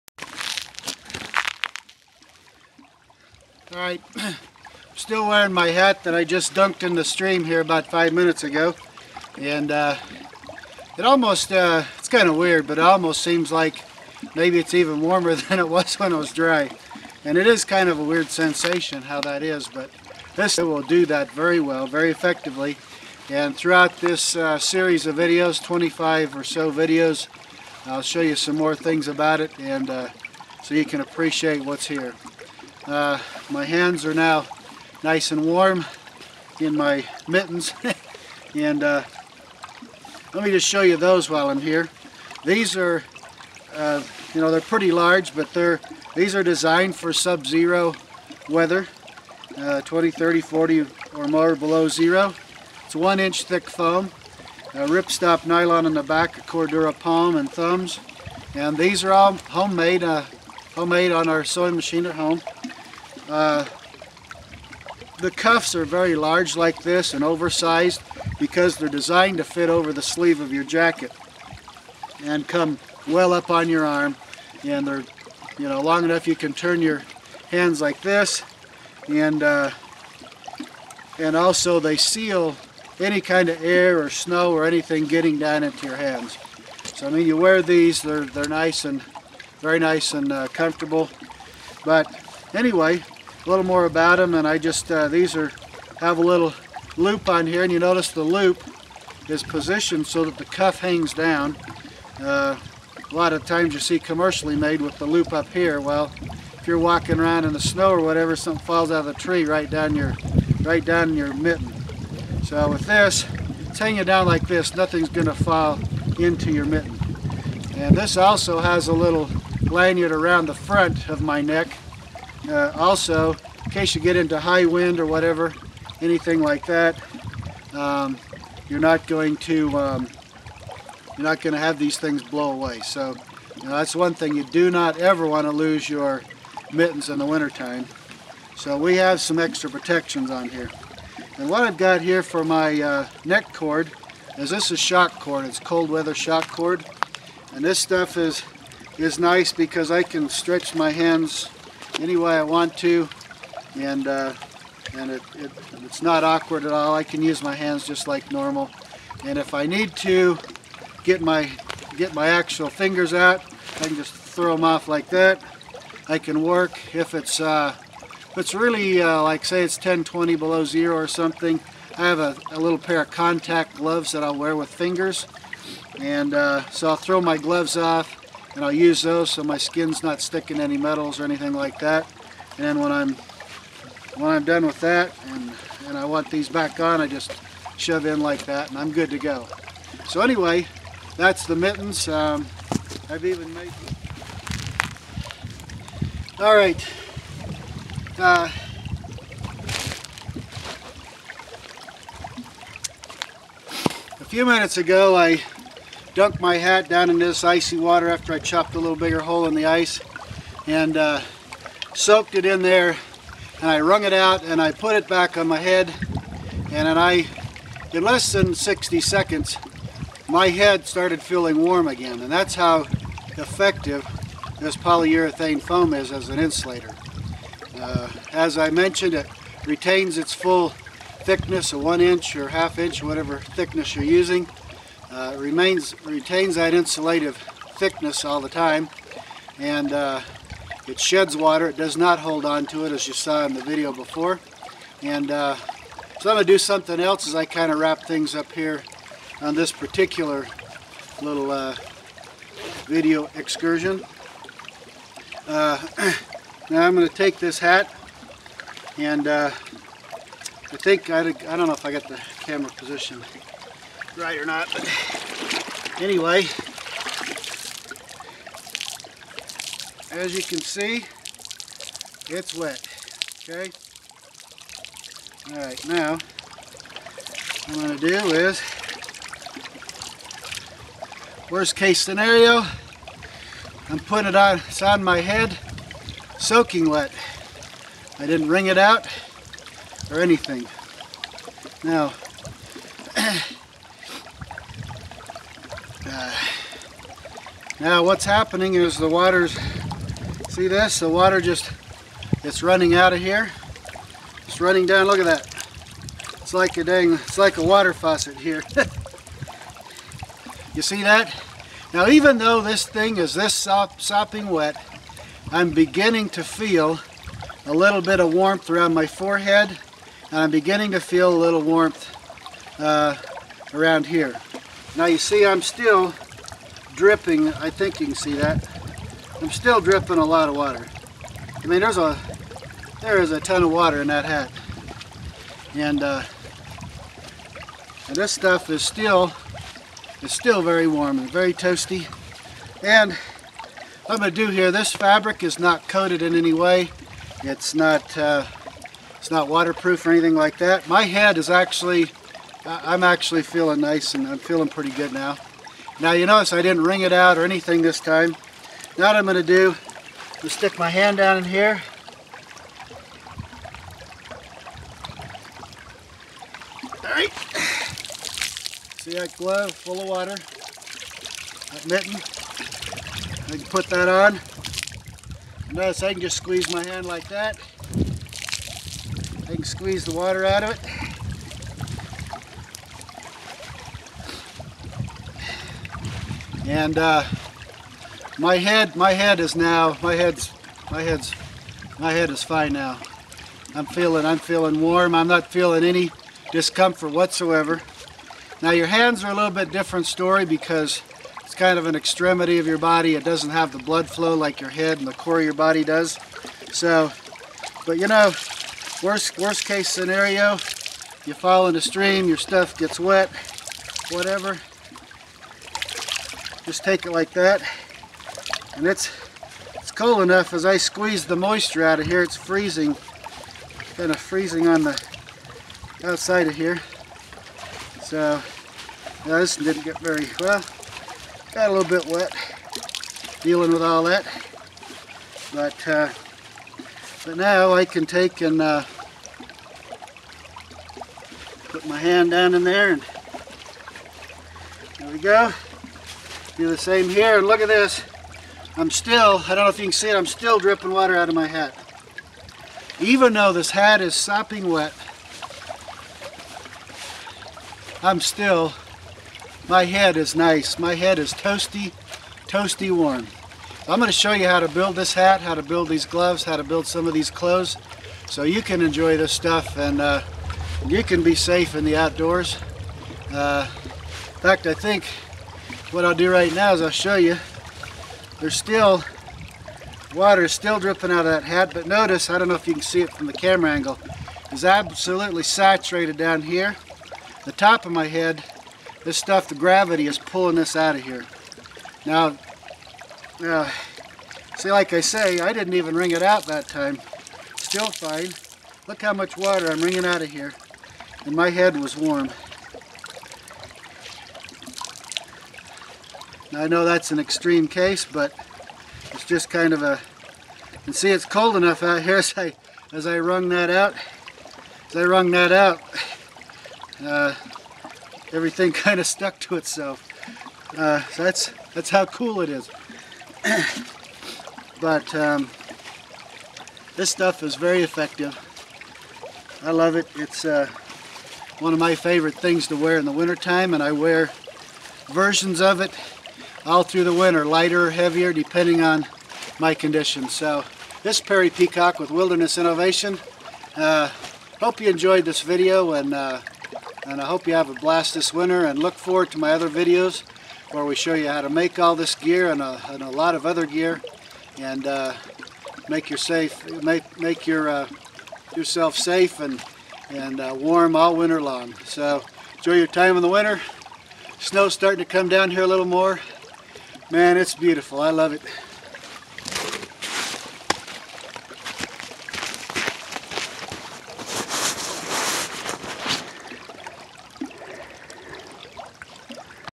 Can mittens and hat be warm when soaked in ice water? watch and see
Part 2 on the foam gear series. Shows the mitten made at home, very warm. Shows both mitten and hat in severe test in icy conditions.